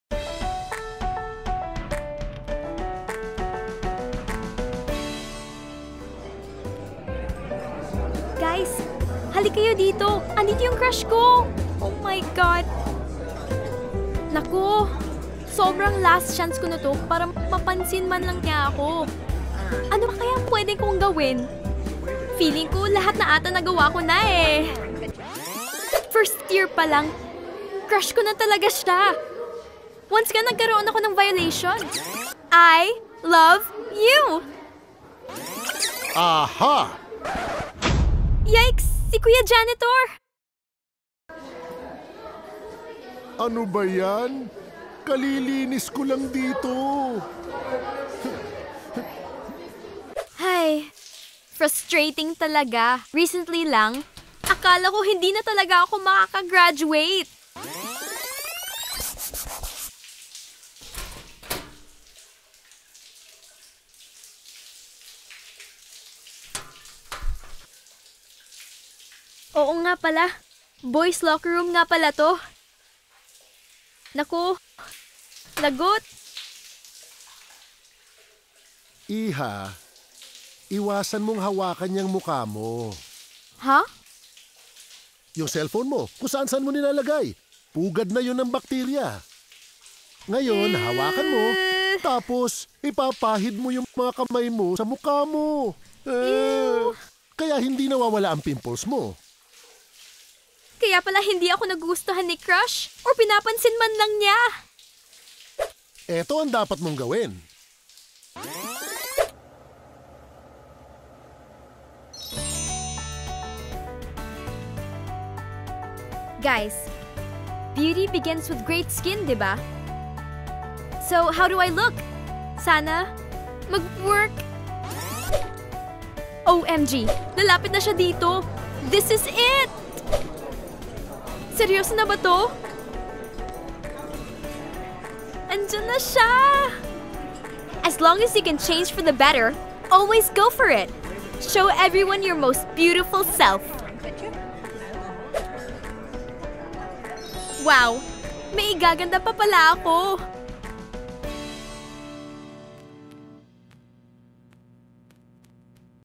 TeenWeekPH 2014 - TransformTuesday (Thea's Beautiful Story)
High School grad party na! Makakausap na ba ni Thea ang long time crush niya?
Let's watch her beautiful story!